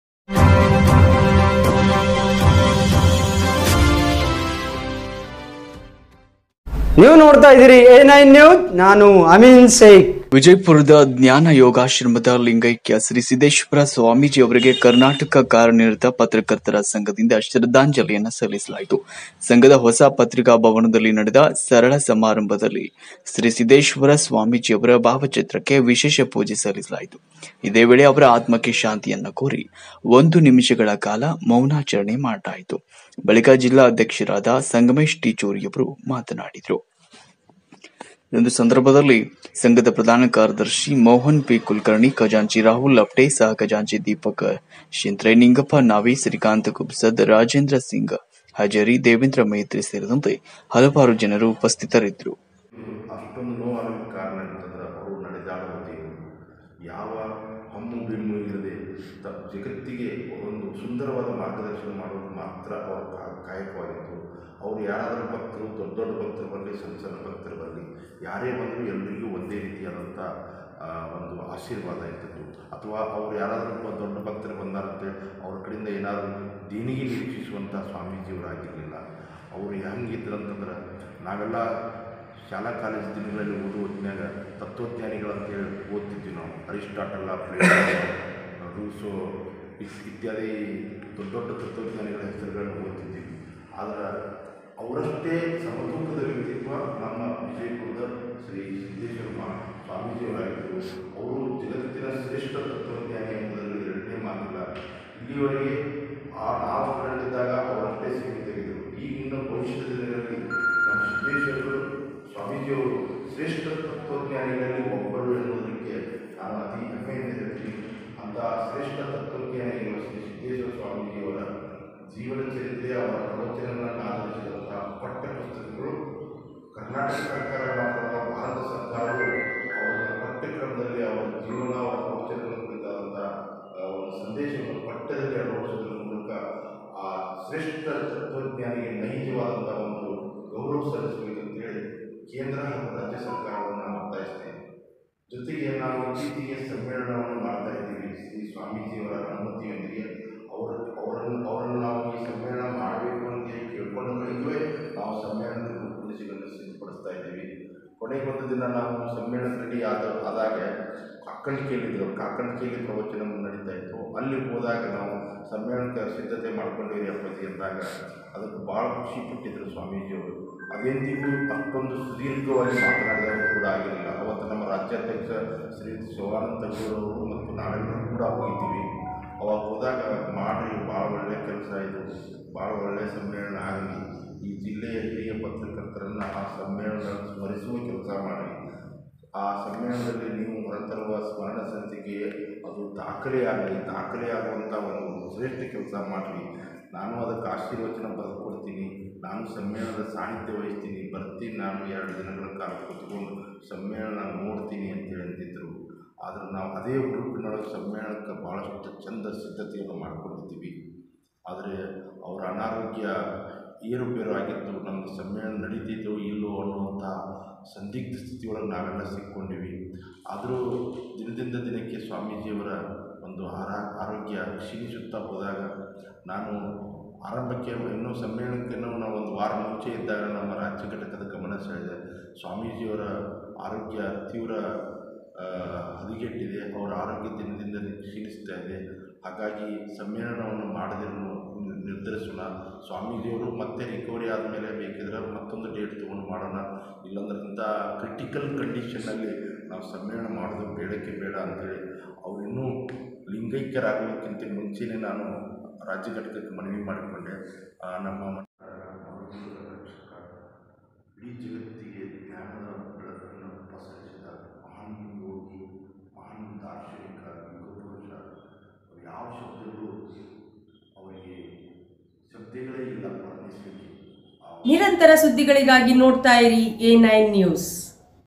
ವಿಜಯಪುರ : ಇಲ್ಲಿನ ಜ್ಞಾನಯೋಗಾಶ್ರಮದ ಲಿಂಗೈಕ್ಯ ಶ್ರೀ ಸಿದ್ದೇಶ್ವರ ಸ್ವಾಮೀಜಿಯವರಿಗೆ A9NEWS BIJAPUR
ವಿಜಯಪುರ : ಇಲ್ಲಿನ ಜ್ಞಾನಯೋಗಾಶ್ರಮದ ಲಿಂಗೈಕ್ಯ ಶ್ರೀ ಸಿದ್ದೇಶ್ವರ ಸ್ವಾಮೀಜಿಯವರಿಗೆ ಕರ್ನಾಟಕ ಕಾರ್ಯನಿರತ ಪತ್ರಕರ್ತರ ಸಂಘದಿAದ ಶ್ರದ್ಧಾಂಜಲಿ ಸಲ್ಲಿಸಲಾಯಿತು.
ಸಂಘದ ಹೊಸ ಪತ್ರಿಕಾ ಭವನದಲ್ಲಿ ನಡೆದ ಸರಳ ಸಮಾರಂಭದಲ್ಲಿ ಶ್ರೀ ಸಿದ್ದೇಶ್ವರ ಸ್ವಾಮೀಜಿ ಅವ ಭಾವಚಿತ್ರಕ್ಕೆ ವಿಶೇಷ ಪೂಜೆ ಸಲ್ಲಿಸಲಾಯಿತು. ಇದೇ ವೇಳೆ ಅವರ ಆತ್ಮಕ್ಕೆ ಶಾಂತಿ ಕೋರಿ ೧ ನಿಮಿಷ ವೌನ ಆಚರಿಸಲಾಯಿತು